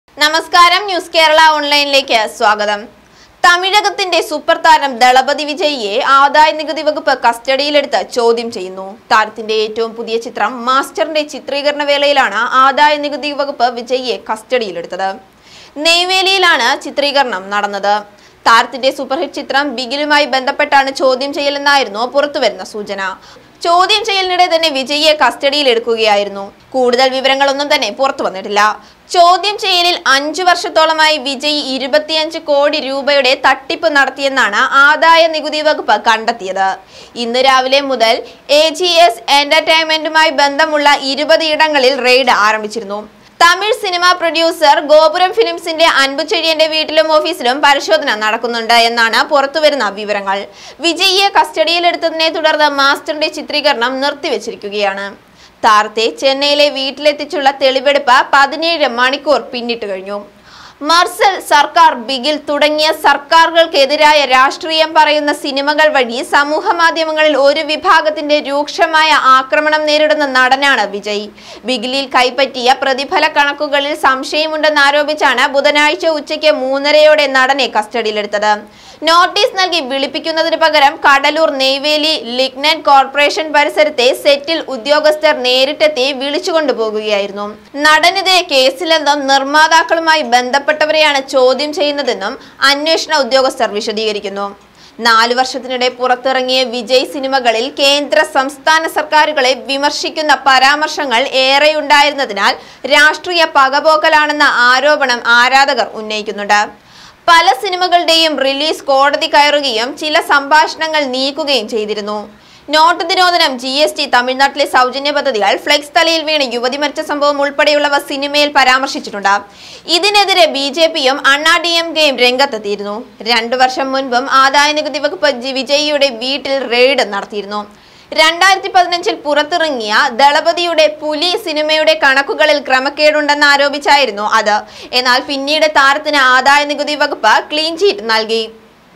Income-Tax raid at Vijay's Chennai house News Kerala Online
ദളപതി വിജയെ ആദായ നികുതി ഉദ്യോഗസ്ഥർ കസ്റ്റഡിയിൽ എടുത്തിട്ട് 17 മണിക്കൂർ പിന്നിടുന്നു